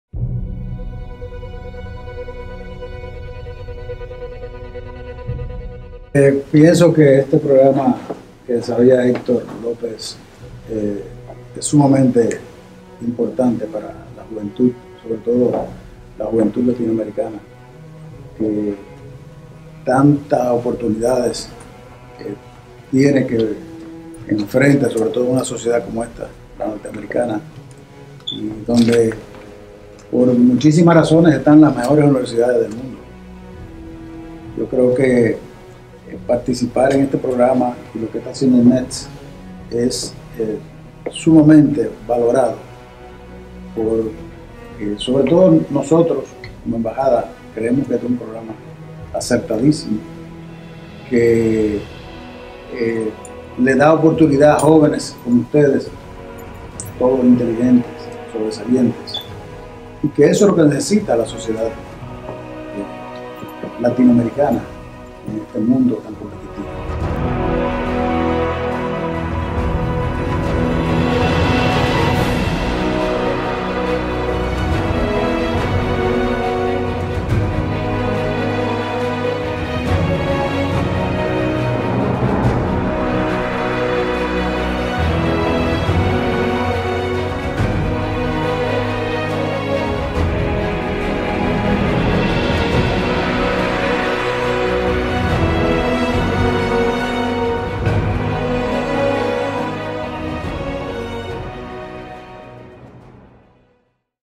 BeNeXT Global | Ambassador José Tomás Pérez
Ambassador Jose Tomas Perez of the Dominican Republic on BeNeXT Global and its programs at Dominican Embassy in Washington.
Wanna listen to the MUSIC featured?